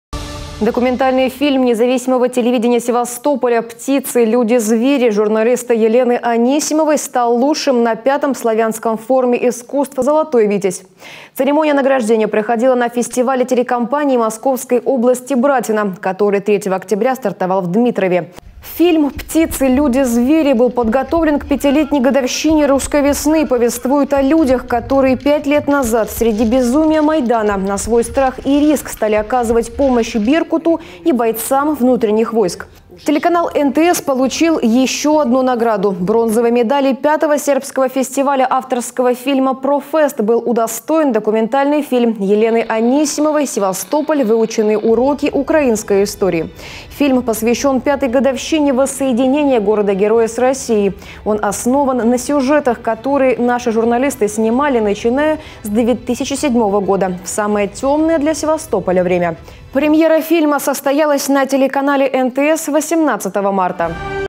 Телеканал НТС получил сразу две награды на фестивале «Братина»
Документальный фильм Независимого телевидения Севастополя «Птицы. Люди. Звери» журналиста Елены Анисимовой стал лучшим на десятом славянском форуме искусств «Золотой витязь». Церемония награждения проходила на фестивале телекомпаний Московской области «Братина», который 3 октября стартовал в Дмитрове. Фильм «Птицы. Люди. Звери» был подготовлен к пятилетней годовщине Русской весны и повествует о людях, которые пять лет назад, среди безумия Майдана, на свой страх и риск, стали оказывать помощь «Беркуту» и бойцам Внутренних войск.